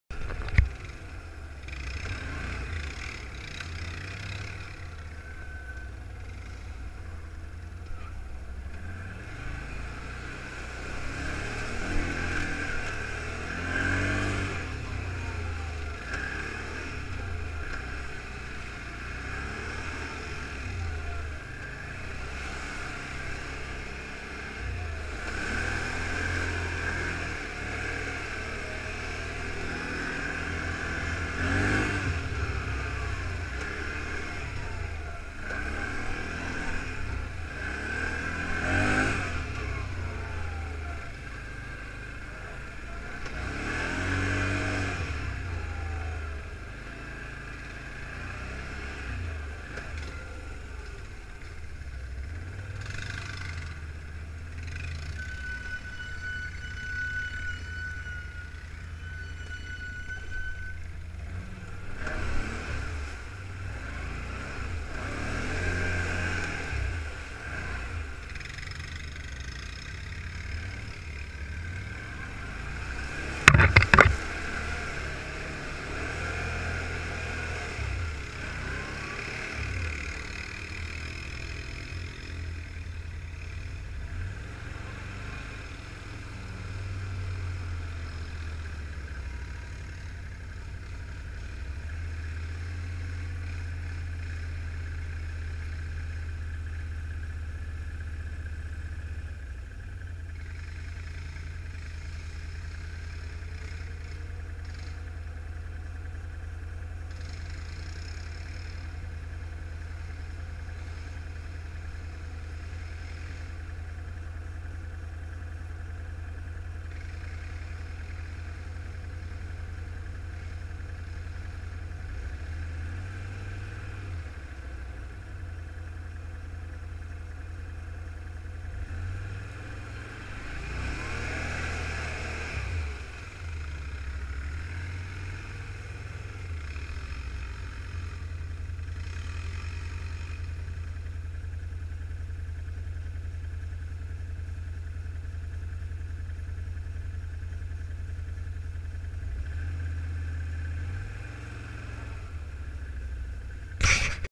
Can Am 500 max xt and Honda Rancher in WF muddin
Testing out my big horns and new winch on the Rancher, but big horns kept diging in the sand too fast. Great in mud, but shovel's in the sand.